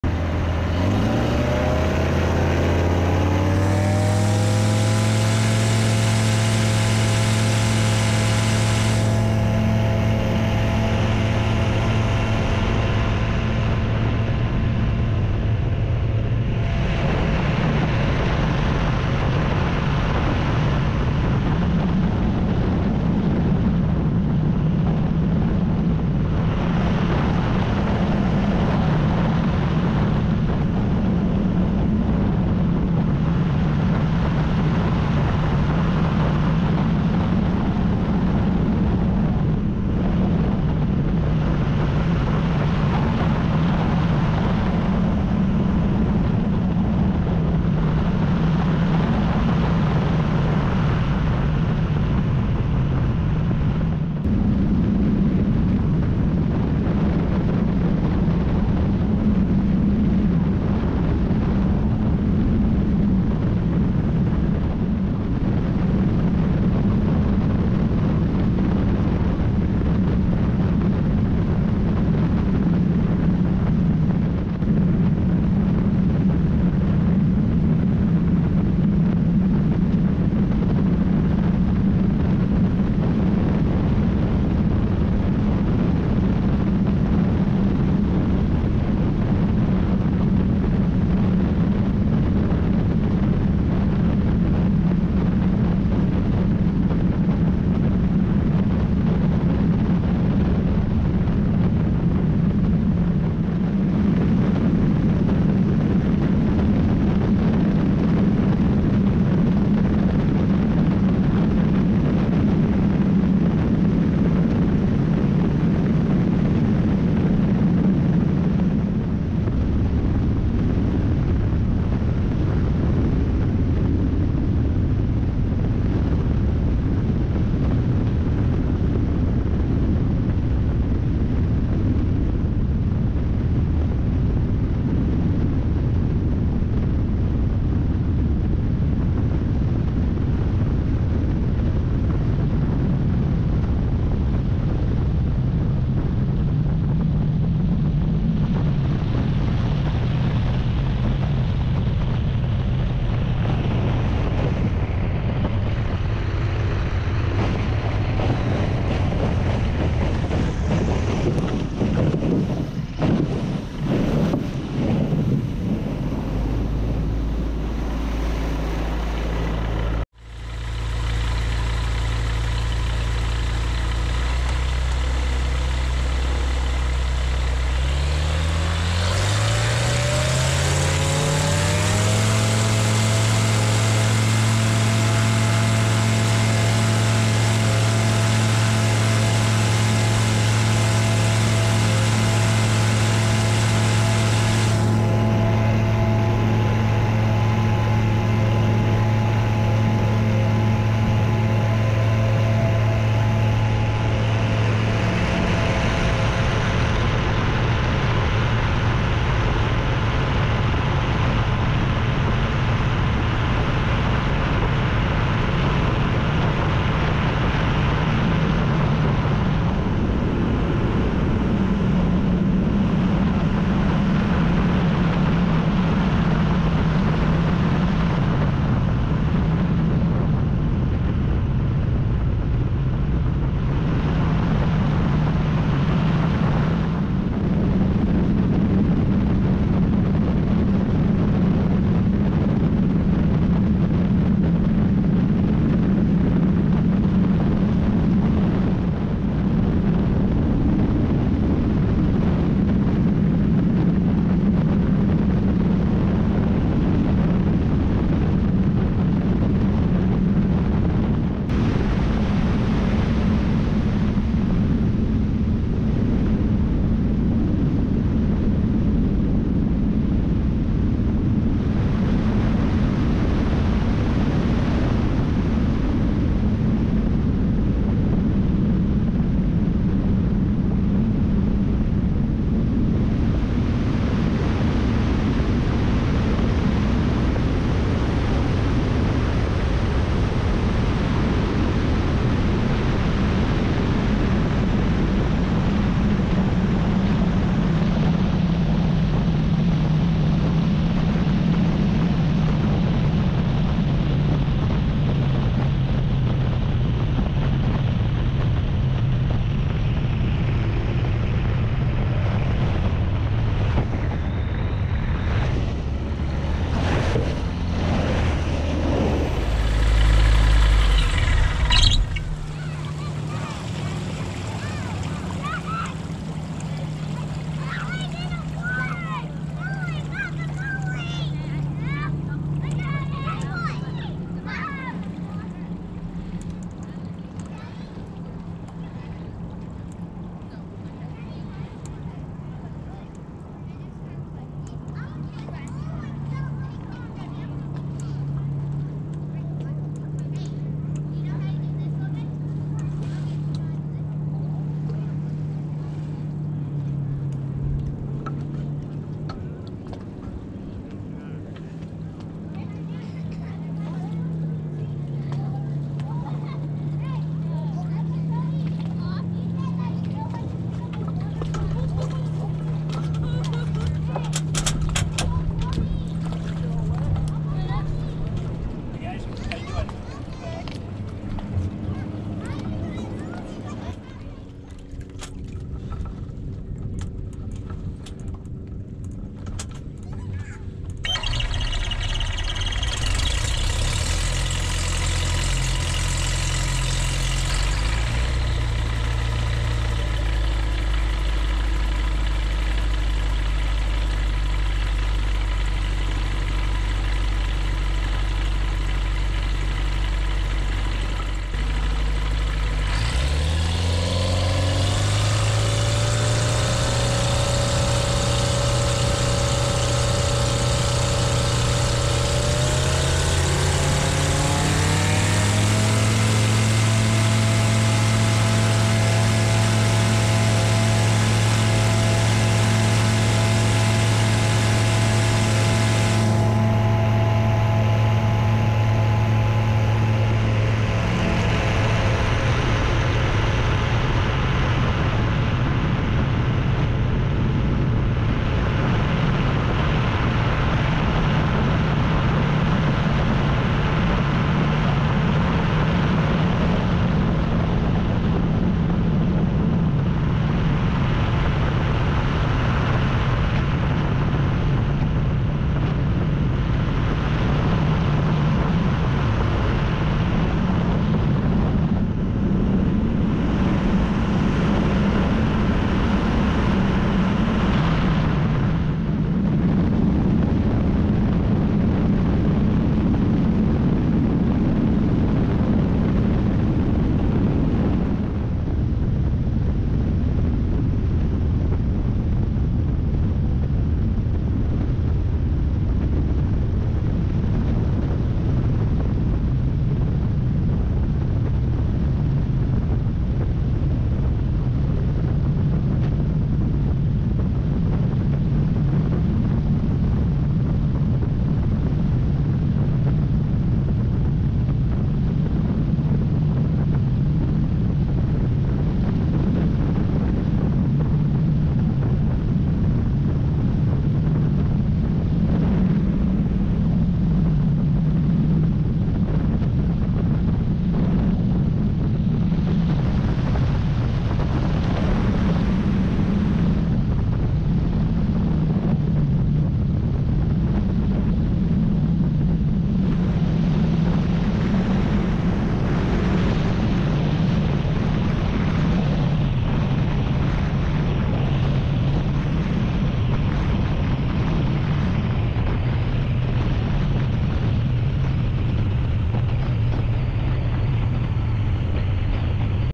Summit Lake and a stop at Mason Lake
A summer afternoon visiting Summit Lake west of Olympia and stopping a Mason Lake near Shelton. Float flying is the best!